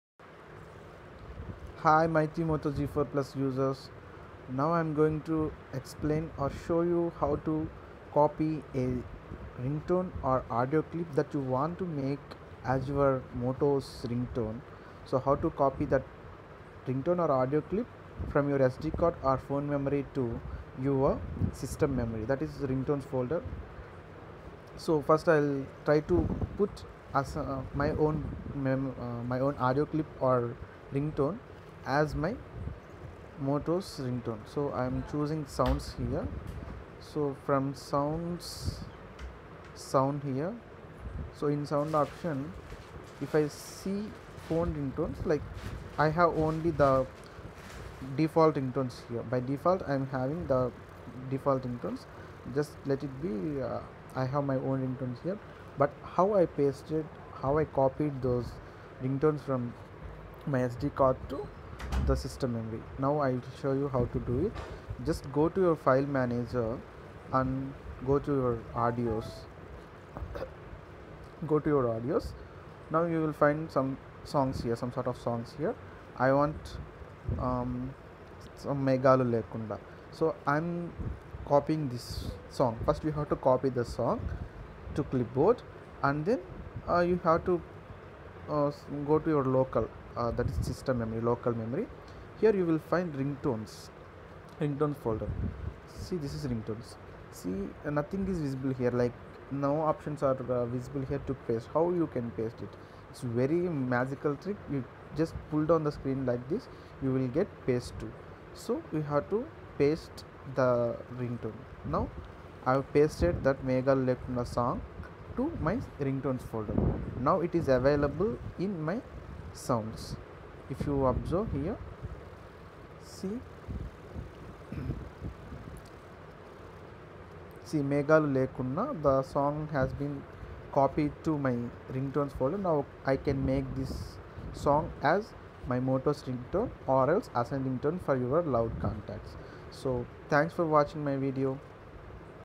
How to set custom ringtone in moto g4 plus
Here I showed you how to make a song that you love as your phone's ringtone.